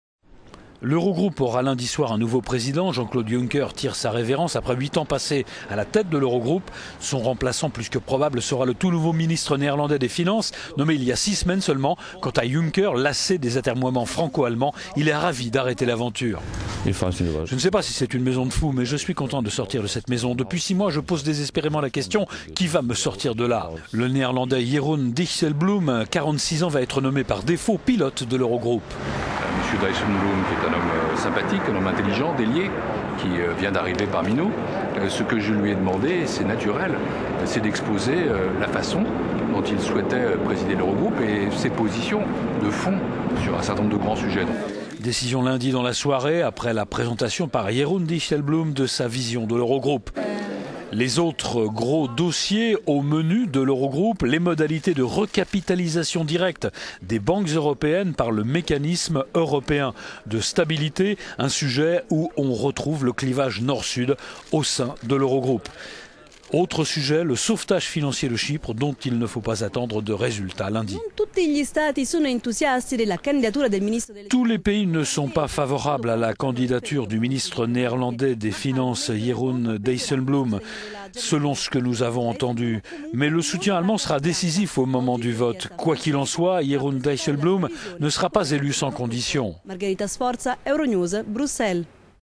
Eurogroupe : un néerlandais novice prend la présidence.
Un des grands sujets abordés par l'Eurogroupe lundi c'est la succession du président Juncker. Le premier ministre luxembourgeois tire sa révérence après 8 ans passés à la tête de l'Eurogroupe. Son remplaçant plus que probable, sera le tout nouveau ministre néerlandais des finances, nommé il y a 6 semaines seulement.

Quand à Juncker, lassé des atermioiements franco-allemand, il est ravi d'arrêter l'aventure. "Je ne sais pas si c'est une maison de fous mais je suis content de sortir de cette maison. Depuis six mois je pose désespérément la question : qui va me sortir de là ?".

Le néerlandais Jeroen Dijsselbloem, 46 ans va être nommé par défaut, pilote de l'Eurogroupe.
"Monsieur Dijsselbloem, qui est un homme sympathique, un homme intelligent, délié, qui vient d'arriver parmi nous. Ce que je lui ai demandé - et c'est naturel - c'est d'exposer la façon dont il souhaitait présider l'Eurogroupe et ses positions de fond sur un certain nombre de grands sujets. "
Décision lundi dans la soirée, après la présentation par Jeroen Dijsselbloem de sa vision de l'Eurogroupe.

Les autres gros dossiers au menu de l'Eurogroupe : les modalités de recapitalisation directe des banques européennes par le Mécanisme européen de stabilité : un sujet oú on retrouve le clivage nord-sud au sein de l'Eurogroupe; et le sauvetage financier de Chypre dont il ne faut pas attendre de résultat lundi.

Tous les pays ne sont pas favorables à la candidature du ministre néerlandais des finances, selon ce que nous avons entendu. Mais le soutien allemand sera décisif au moment du vote. Quoiqu'il en soit, Jeroen Dijsselbloem ne sera pas élu sans conditions.